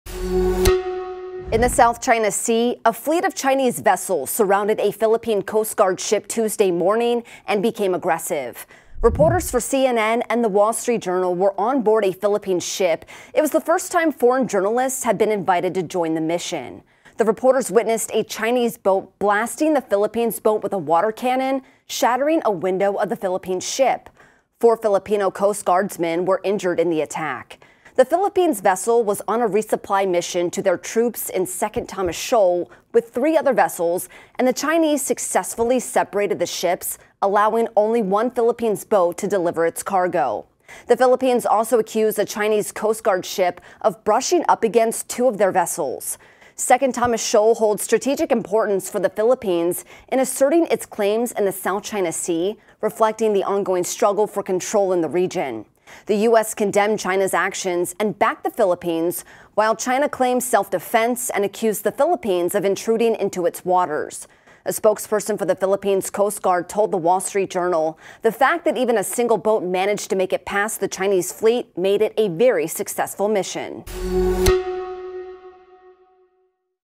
Chinese fleet surrounds Philippine vessels in South China Sea
In the South China Sea, a fleet of Chinese vessels surrounded a Philippine Coast Guard ship Tuesday morning, March 5. CNN and The Wall Street Journal reporters were aboard a Philippine ship, marking the first instance of foreign journalists joining the mission. The reporters observed a Chinese boat firing a water cannon at the Philippine vessel, shattering one of its windows.